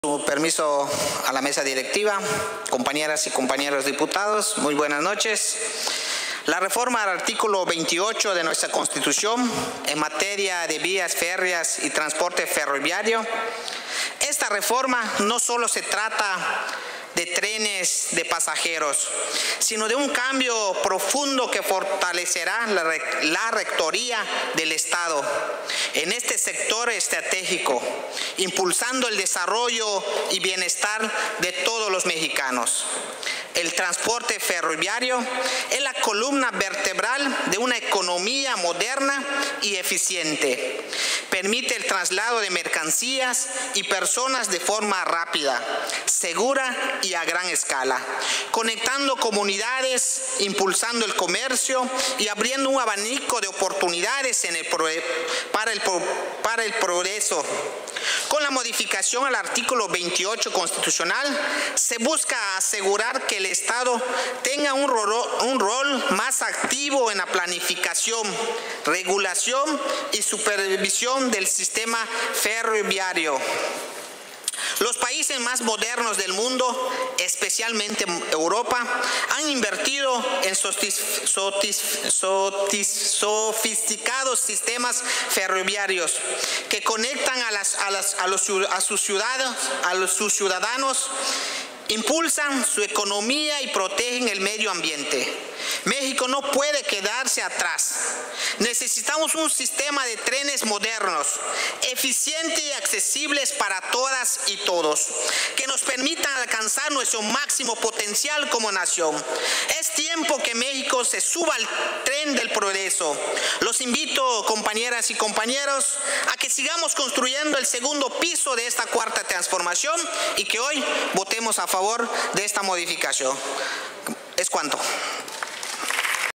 Intervención Dip. José María Chacón Chablé. MORENA (21/Octubre/2024)
Intervención del Dip. José María Chacón Chablé, de MORENA, en relación a la Minuta con Proyecto de Decreto por el que se reforma el párrafo cuarto y se adiciona un párrafo quinto, al artículo 28 de la Constitución Política de los Estados Unidos Mexicanos, en materia de Vías y Transporte Ferroviario; remitida por la Cámara de Senadores del H. Congreso de la Unión.